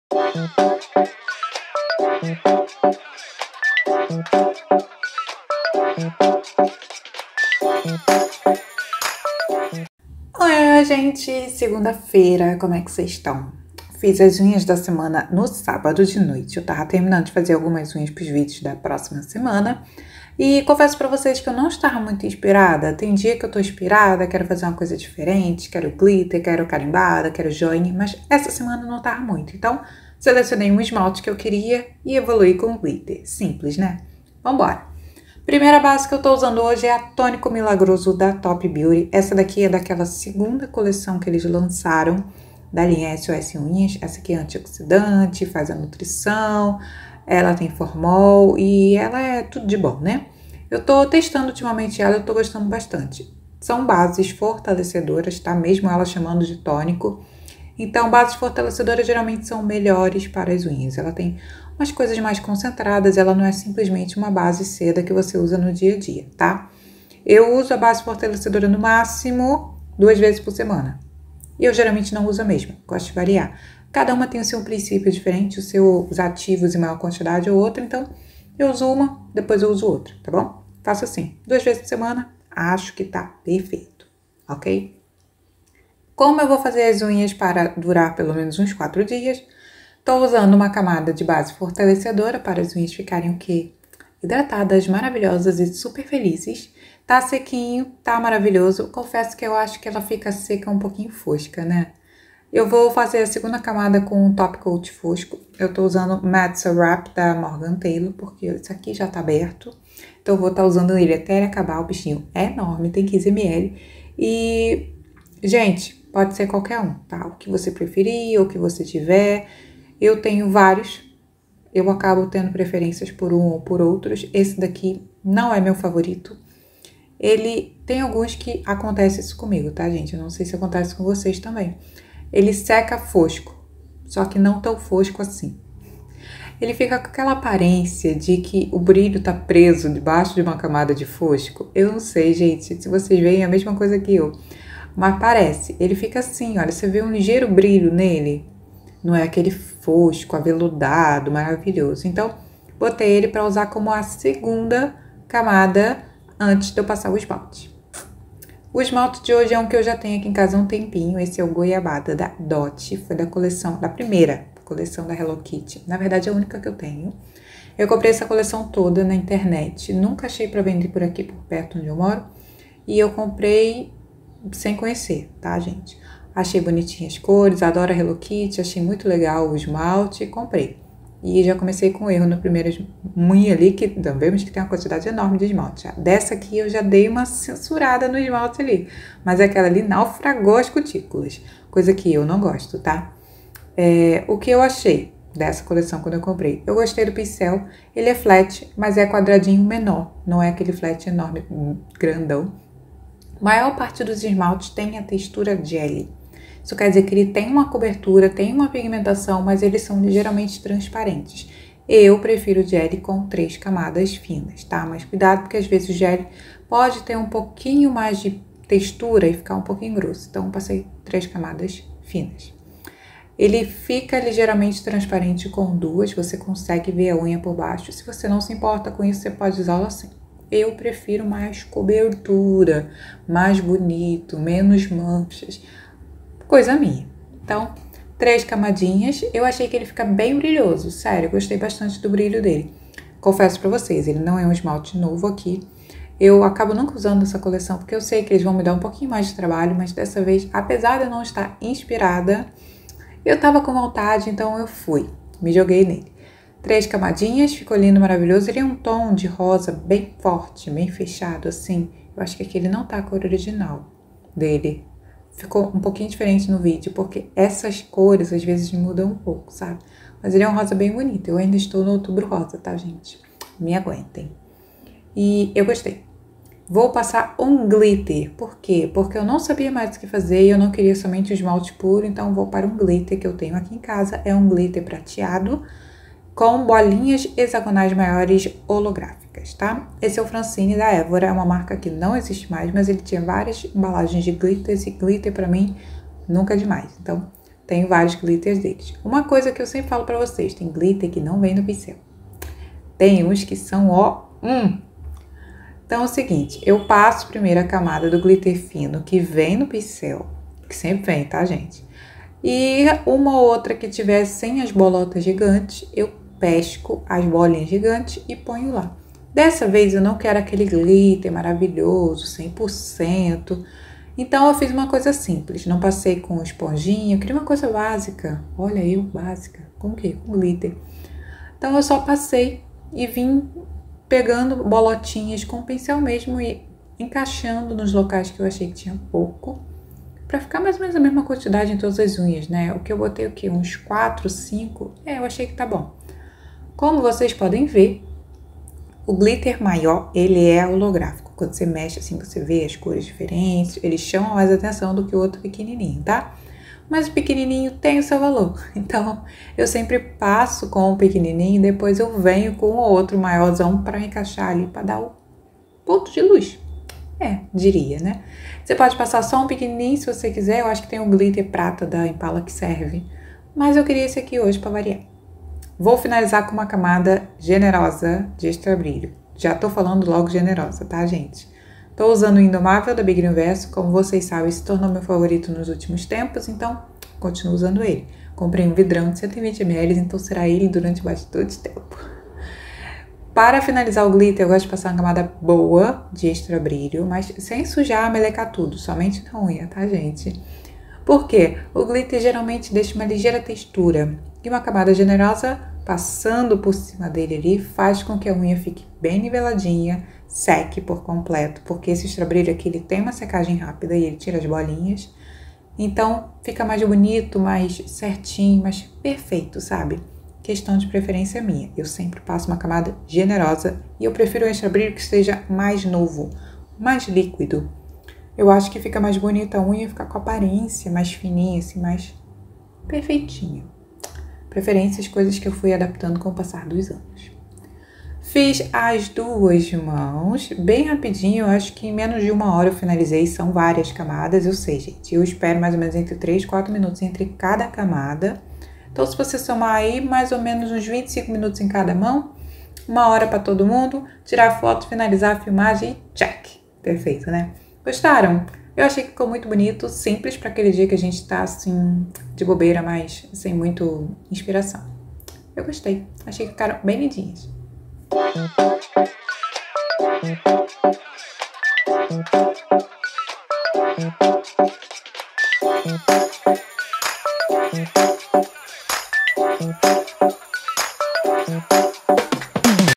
💅🏻 Unhas da Semana - Rosa com Brilho | Unhas da Marie
Trouxe um quadro novo e mais um dia de postagens pra vocês que espero que gostem assim como eu.

Esmalte da Semana: Goiabada da Dote

Me siga no Instagram, tem postagens diárias por lá!

Gostou do vídeo, me deixa um alô nos comentários e se tiver sugestões para novos vídeos!